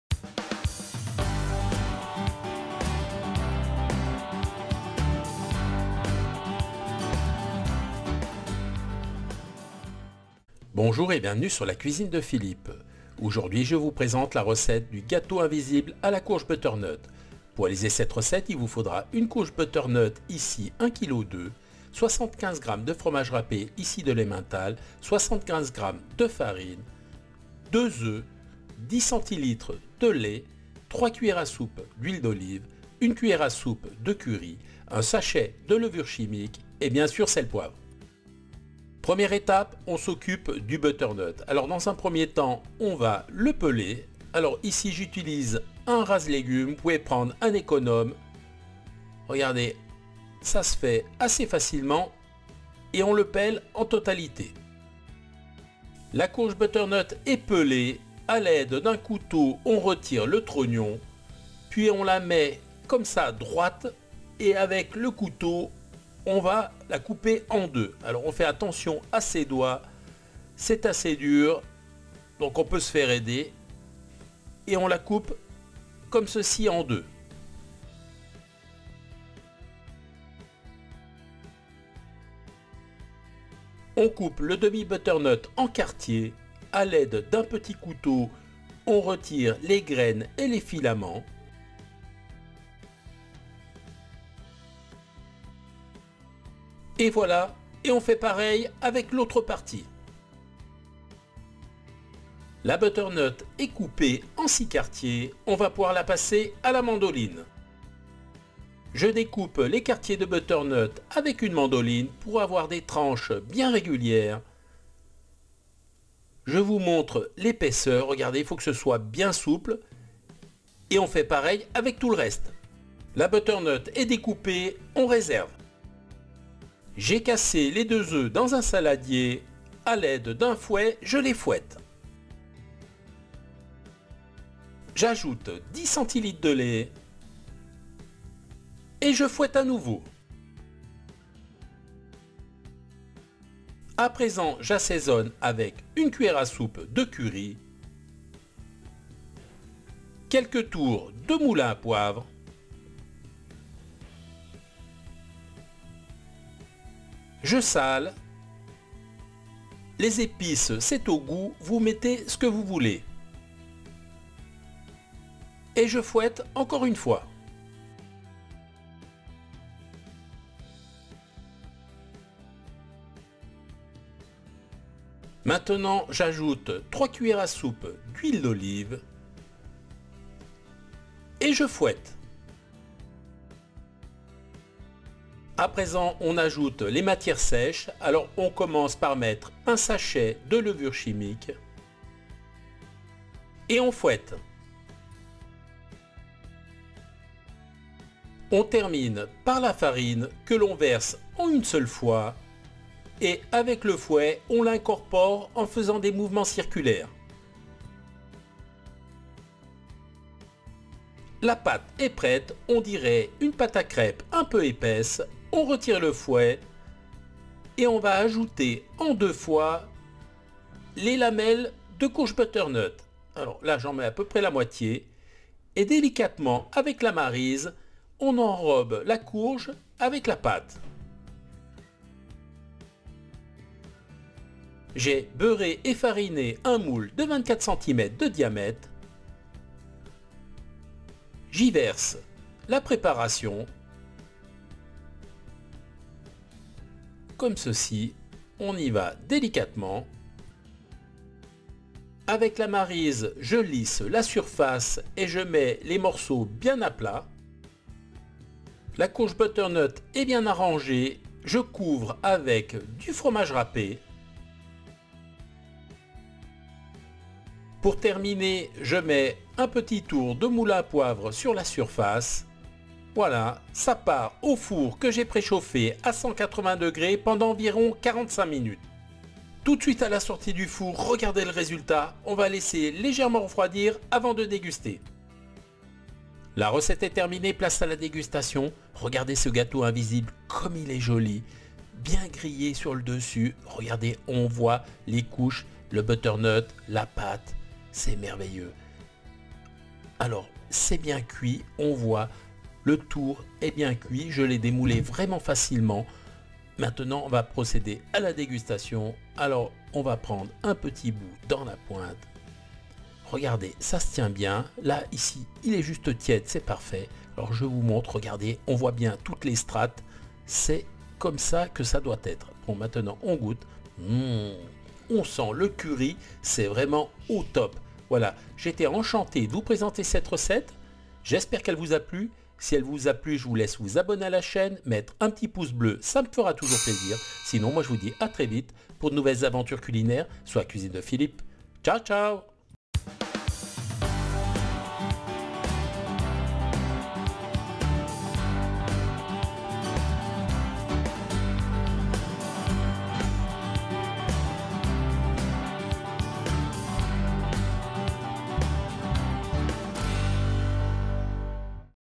Gâteau invisible à la courge butternut
Les courges fleurissent sur les étals et j'avais envie de mettre en avant la butternut dans une version de gâteau invisible à la courge butternut.
Le concept du gâteau invisible consiste à couper finement un légume et à l'incorporer dans une pâte puis de tout mettre en cuisson au four. A la découpe, on voit les différentes strates de butternut qui alternent avec les strates de pâte.
Dans cette recette, j'ai parfumé les mon gâteau invisible avec du curry qui vient réveiller le côté douceâtre de la courge. Cependant, on peut utiliser d'autres épices selon ses goûts et envies.
Le secret de la réussite se trouve dans la découpe de la courge butternut, ici j'ai utilisé une mandoline mais on peut aussi faire ça au couteau ou encore avec un robot.
Le gâteau invisible à la courge butternut se déguste chaud ou froid mais j'avoue que personnellement je l'adore juste tiède avec quelques feuilles de salade verte.
Comme vous pouvez le voir il existe de multiples façons de cuisiner cette courge et le gâteau invisible est vraiment une version à tester.
Alors prêt à vous lancer dans cette nouvelle aventure ?
Si oui, vite à vos tabliers !
Ingrédients :
1 courge butternut (env 1,2kg)
75 gr de fromage râpé
75 gr de farine
10 cl de lait
2 œufs
1 cuillère à soupe de curry
 3 cuillères à soupe d'huile d'olive
1 sachet de levure chimique
Sel/poivre

Temps de cuisson : 45 min au four à 180°C

vous pouvez soutenir la chaîne en faisant